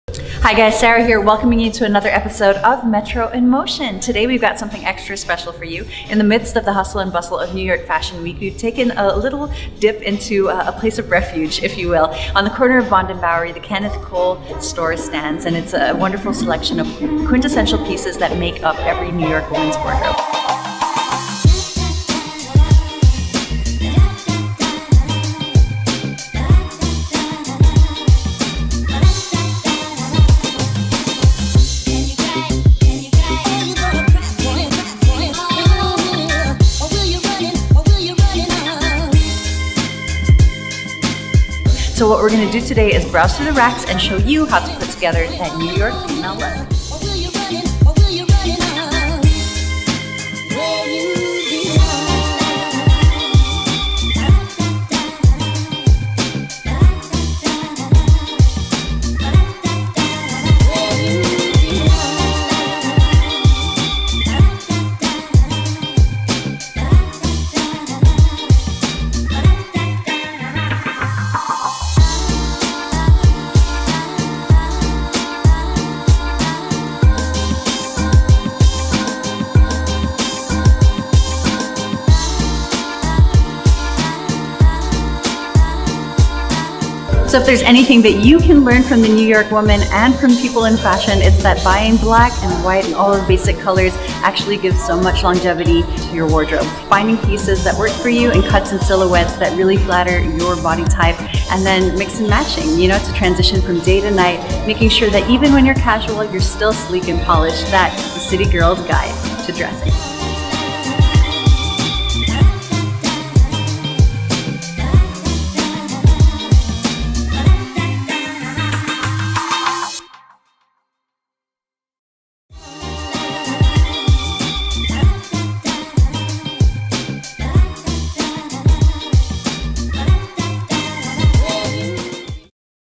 Metro In Motion | Style Shop At Kenneth Cole
How to dress-up like a New York woman? 'Getting a quick break from all that Fashion Week buzz, as we do a little shopping in one of our favorite uptown stores, Kenneth Cole.

Produced by Eldzs Mejia
Video by Fold Canela